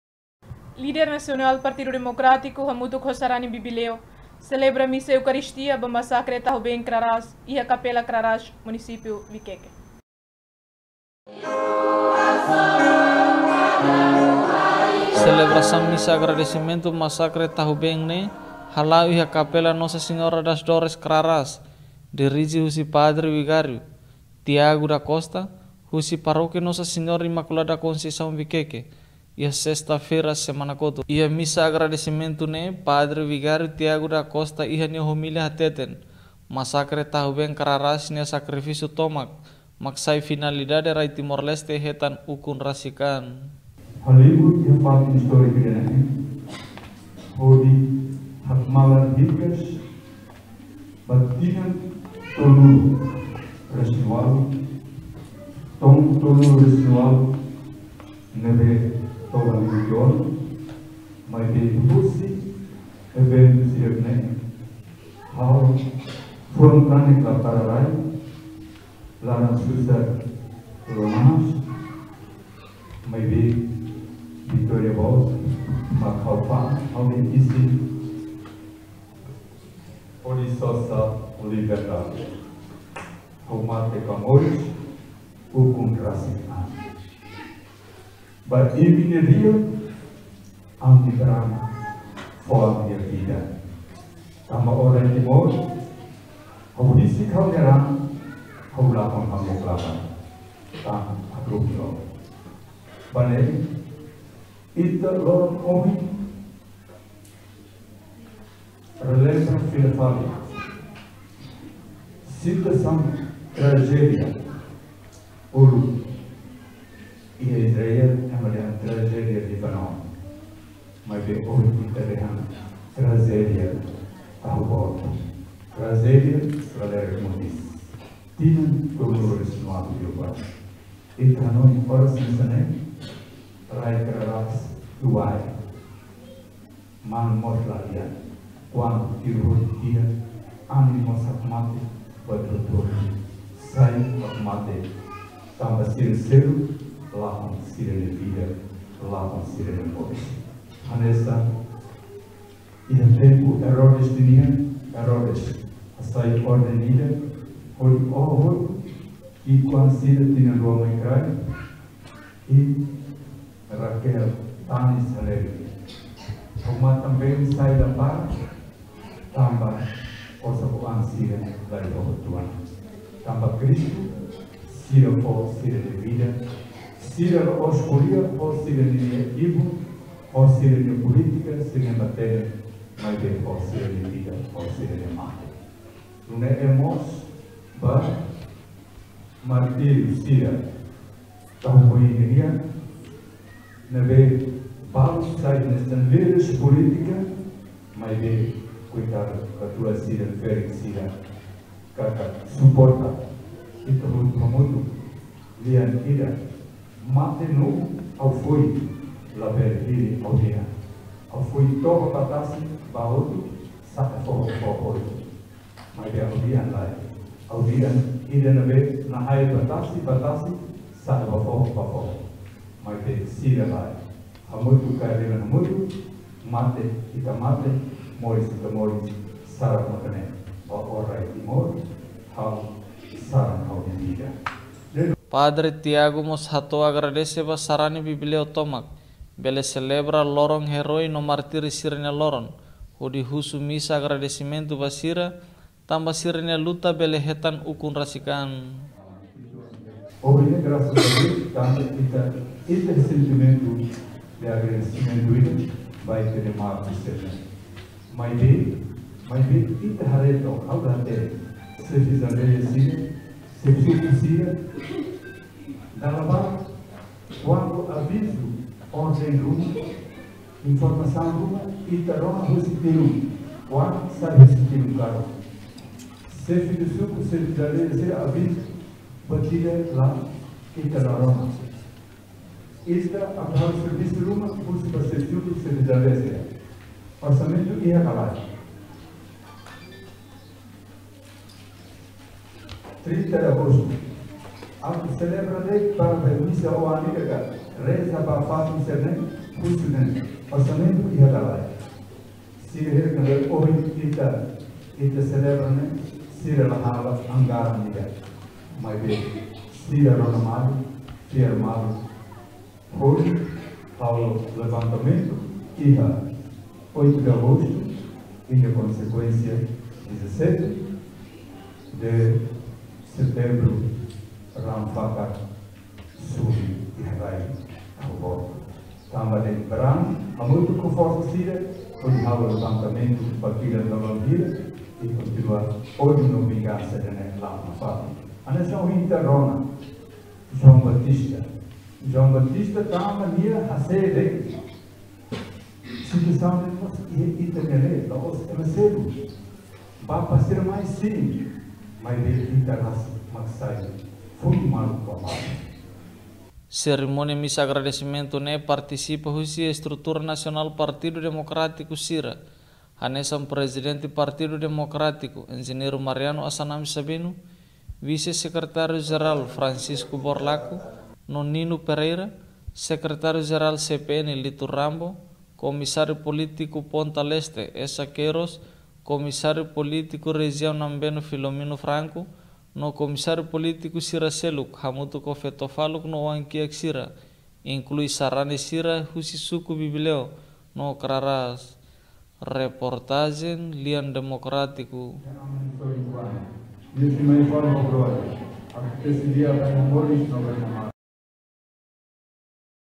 PD Hamutuk ho Familia Masakre Tahu-Ben Selebra Misa Masakre Tahu-Ben Iha Kapela Kráras
Estrutura Nasional PD ho Estrutura PD Munisipiu Viqueque Selebra Misa ba Masakre Tahu-Ben Iha Kapela Kráras